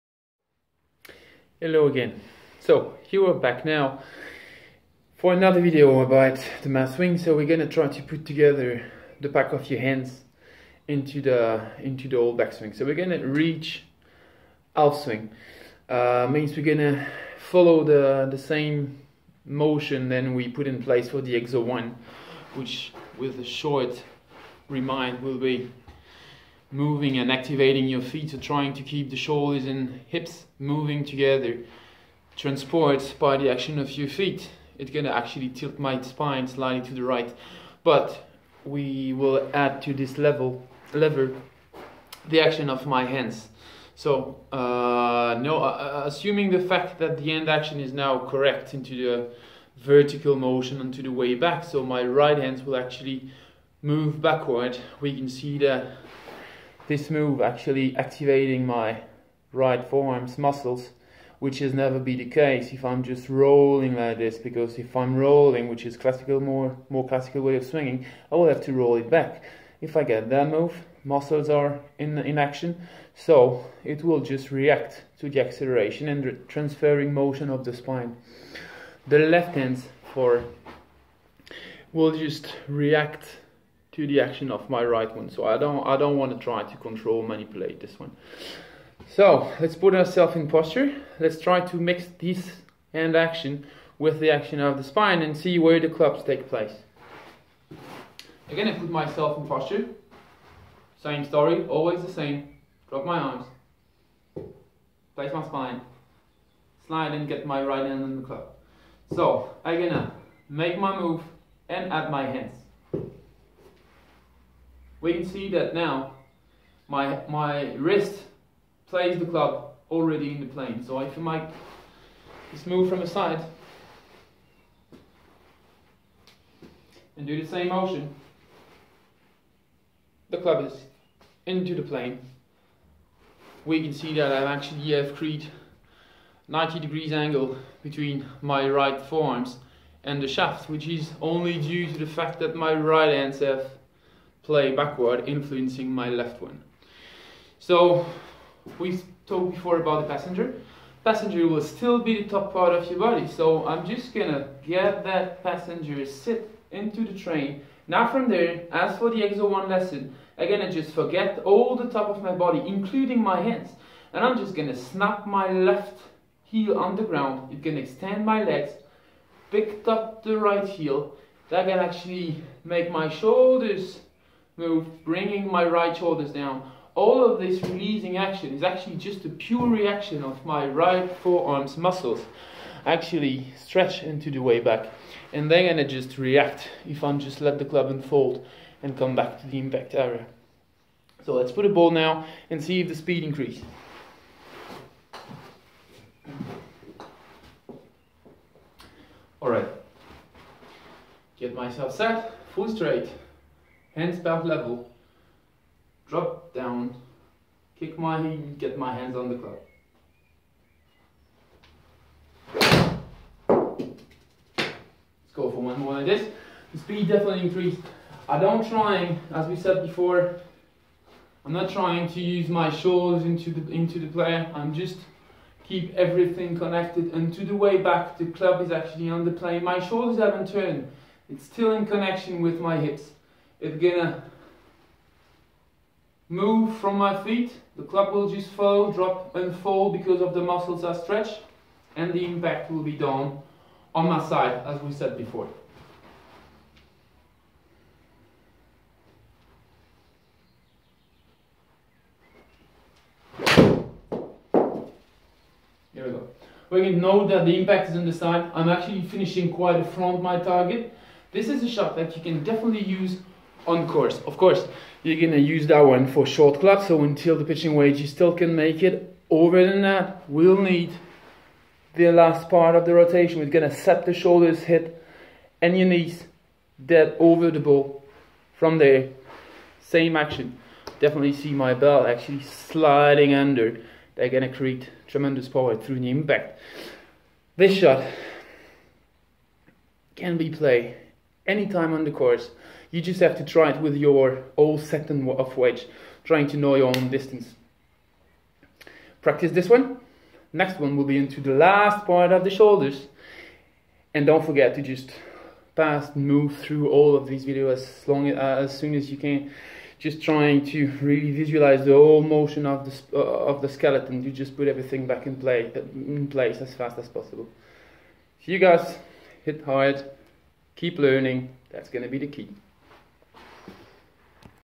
Olivier schmit Perfectswing Exo3
Complete the top part lever connection and reach à powerfull alf swing position